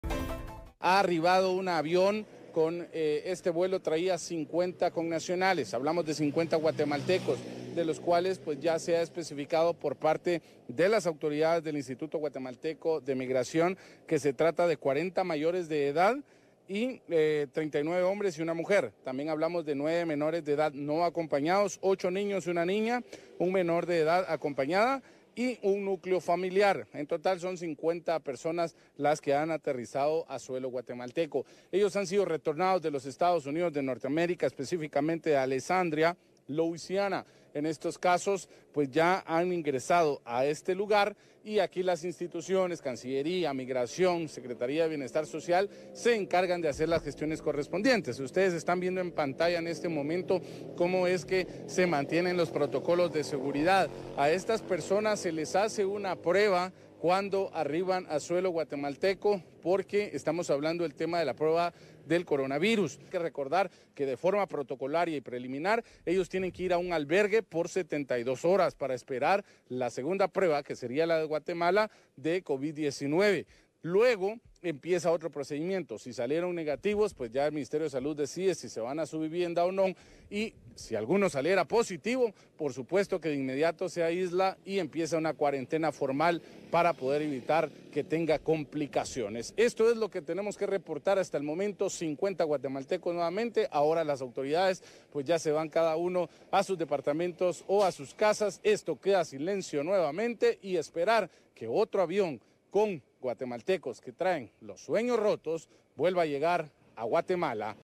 Arribó vuelo con 50 connacionales desde los EE.UU.
A todos se les practicó la prueba Covid-19

Durante la tarde de este martes 9 de junio llegaron al país, 50 connacionales provenientes de los Estados Unidos. Todos fueron sometidos a la prueba de Covid-19.